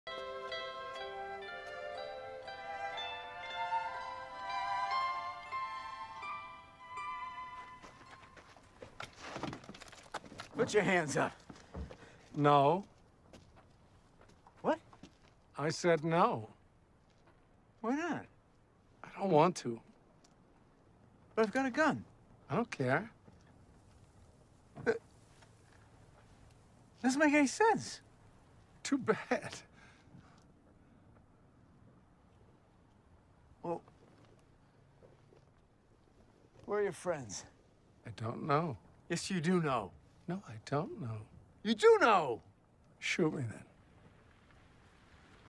Alpharius and Rogal Dorn talking in the Battle of Pluto
Rogal Dorn says no meme! Quite possibly the lowest quality meme I've made... enjoy!!